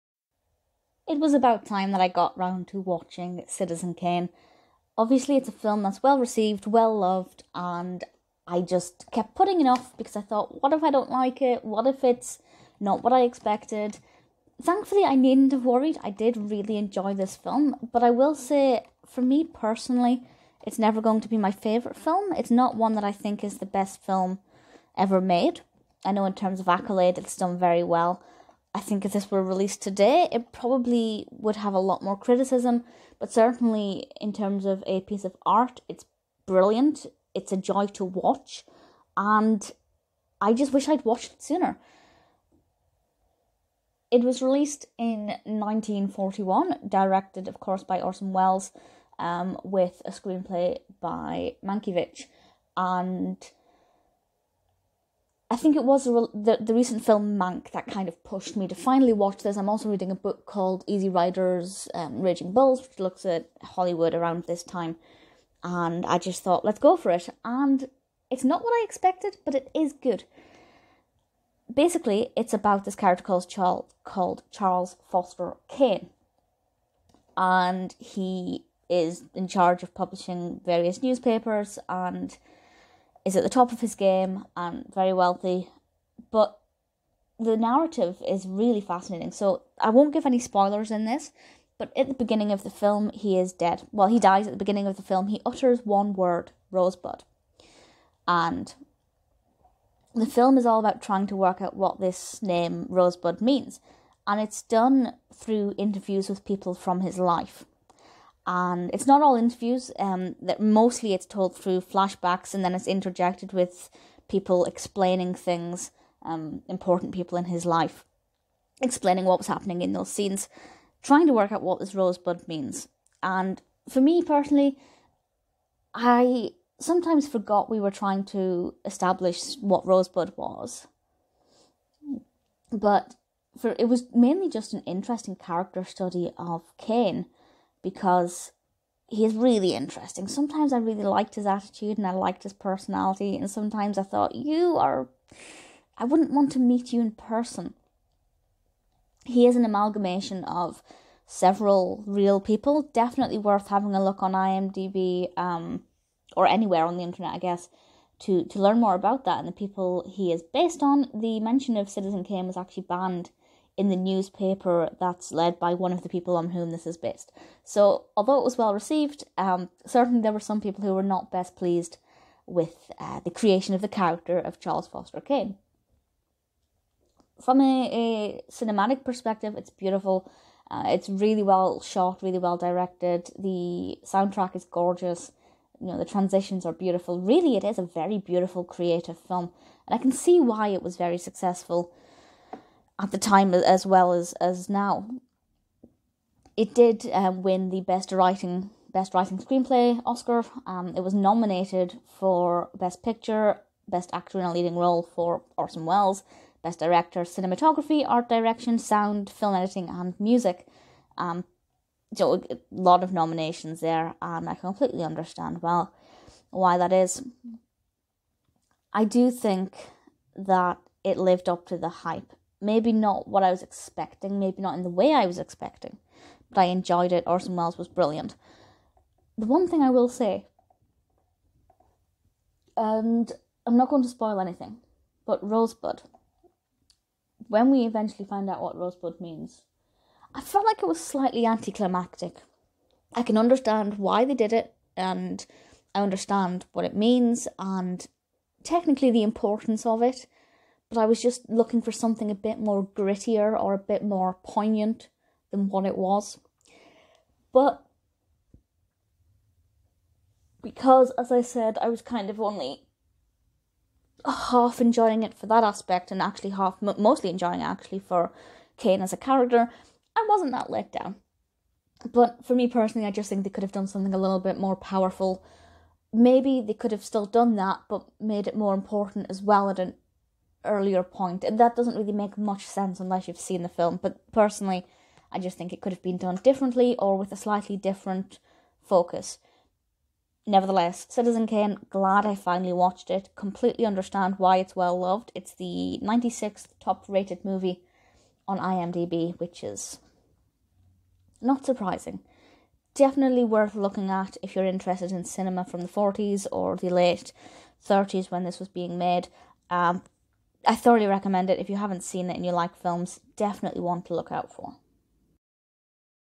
Citizen Kane 1941 Film | Orson Welles | Review
MY BOOKS What's your review of the 1941 film Citizen Kane? It's directed by Orsol Welles, and is written by Herman J. Mankiewicz. [OPEN FOR LINKS AND FACTS!] CitizenKane OrsonWelles Mankievicz Do you want to watch Citizen Kane? Did you enjoy the full movie or just parts of it? Scroll down for more information! What do you think of the Citizen Kane rosebud meaning?

Amy McLean

ABOUT THE ORSON WELLES CITIZEN KANE MOVIE

Citizen Kane plot from IMDb: "Following the death of publishing tycoon Charles Foster Kane, reporters scramble to uncover the meaning of his final utterance; 'Rosebud'."

Directed by Orson Welles
Produced by Orson Welles
Screenplay by Herman J. Mankiewicz, Orson Welles
Cinematography: Gregg Toland
Edited by Robert Wise
Production company: Mercury Productions
Distributed by RKO Radio Pictures
Release date: May 1, 1941 (Palace Theatre), September 5, 1941 (United States)
Budget: $839,72
Box office: $1.6 million (re-releases)

Cast and Characters:
Joseph Cotten as Jedediah Leland
Dorothy Comingore as Susan Alexander Kane
Agnes Moorehead as Mary Kane
Ruth Warrick as Emily Monroe Norton Kane
Ray Collins as Jim W. Gettys
Erskine Sanford as Herbert Carter
William Alland as Jerry Thompson
Paul Stewart as Raymond
George Coulouris as Walter Parks Thatcher
Fortunio Bonanova as Signor Matiste
Gus Schilling as John, headwaiter at the El Rancho nightclub
Georgia Backus as Bertha Anderson
Harry Shannon as Jim Kane
Sonny Bupp as Charles Foster Kane III
Buddy Swan as Charles Foster Kane
Orson Welles as Charles Foster Kane